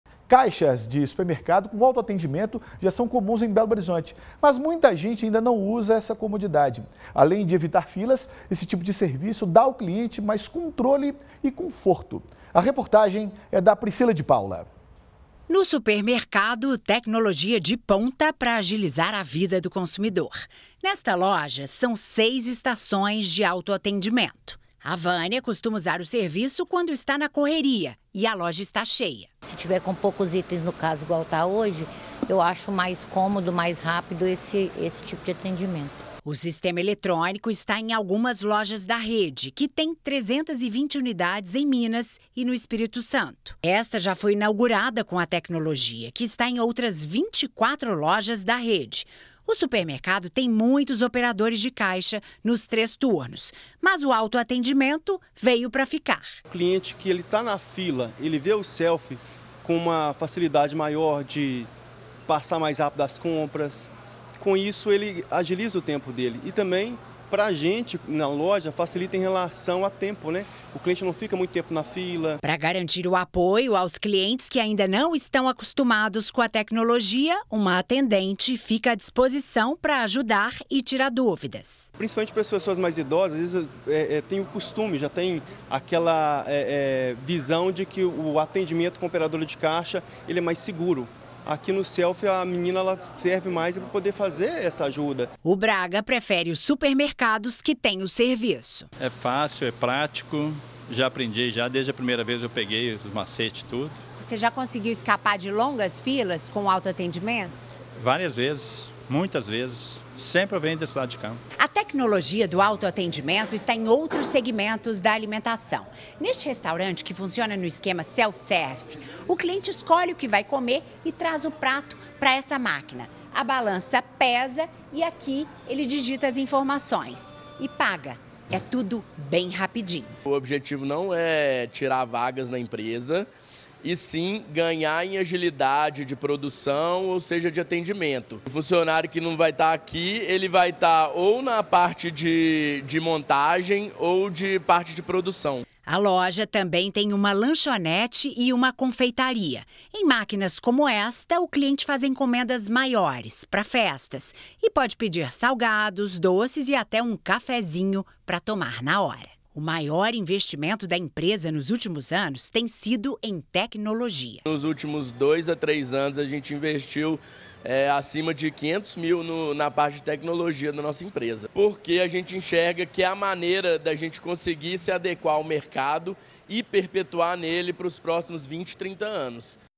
Caixas de autoatendimento aumentam comodidade dos clientes
Caixas de supermercado com autoatendimento já são comuns em Belo Horizonte, mas muita gente ainda não usa essa comodidade. Além de evitar filas, esse tipo de serviço dá ao cliente mais controle e conforto. A tecnologia foi tema da segunda reportagem da série sobre atacado e varejo, exibida nesta terça-feira (15).